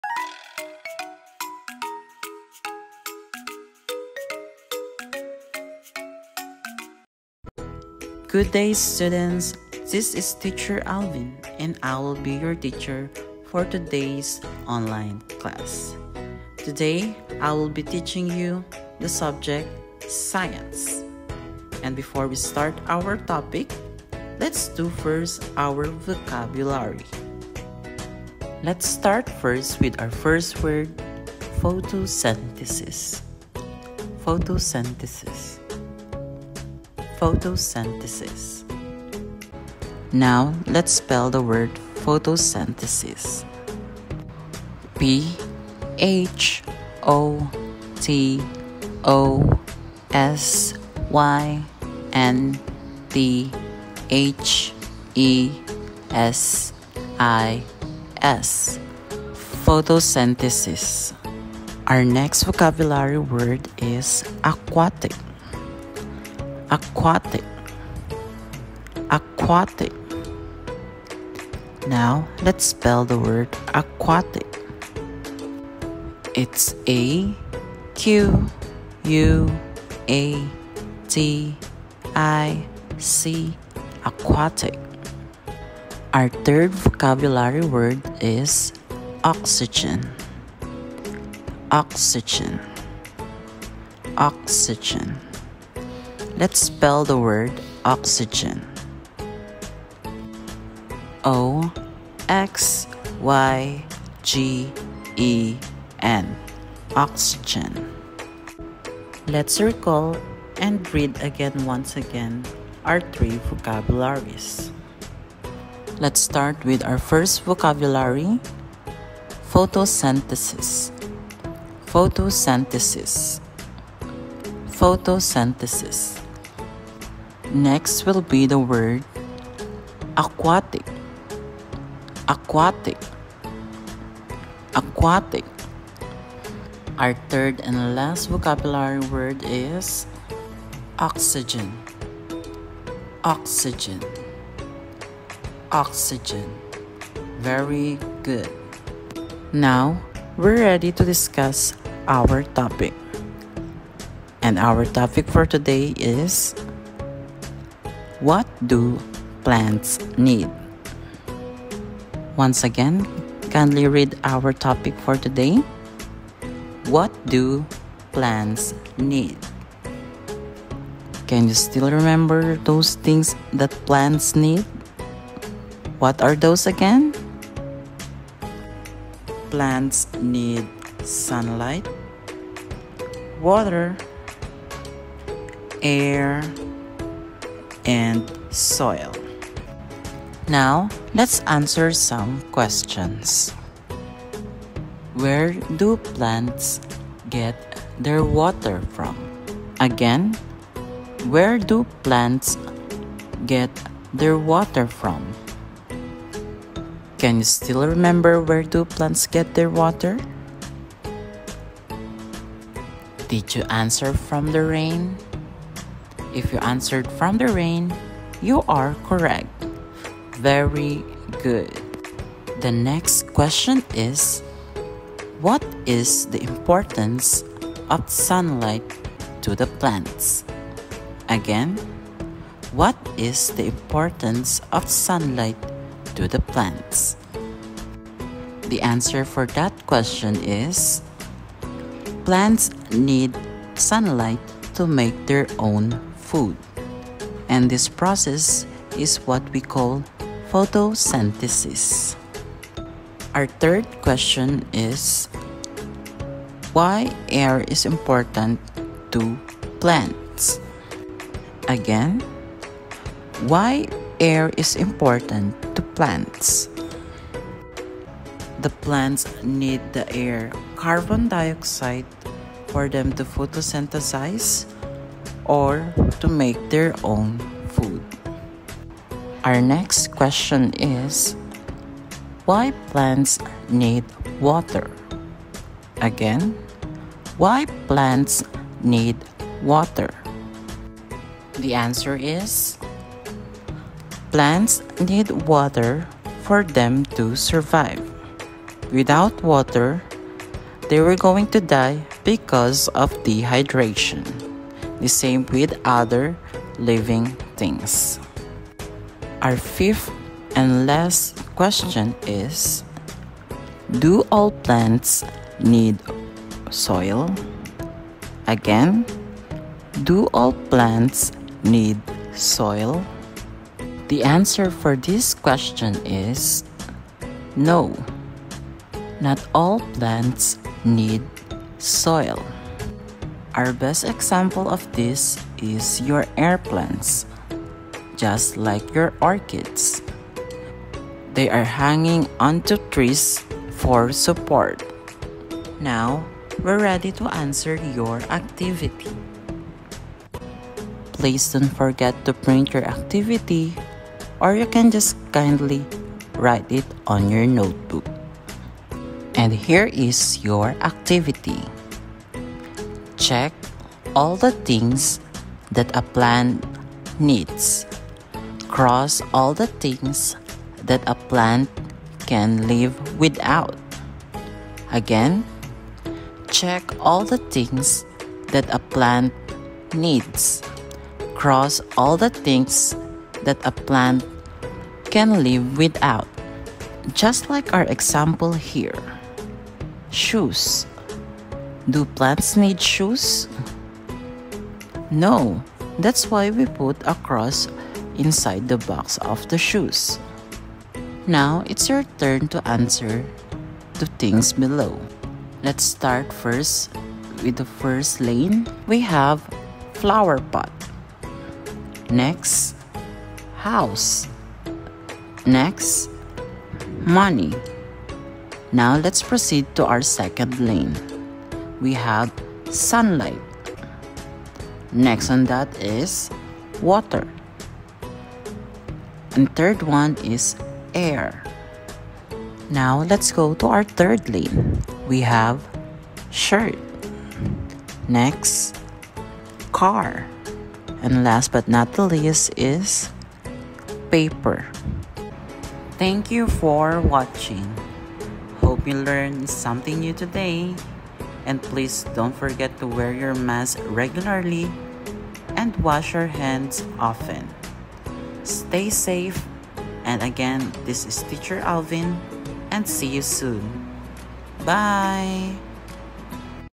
Science for grade 2 Part 4 (What fo plants need?)
Online class for grade 2 students
Part 4
Science subject
What do plants need?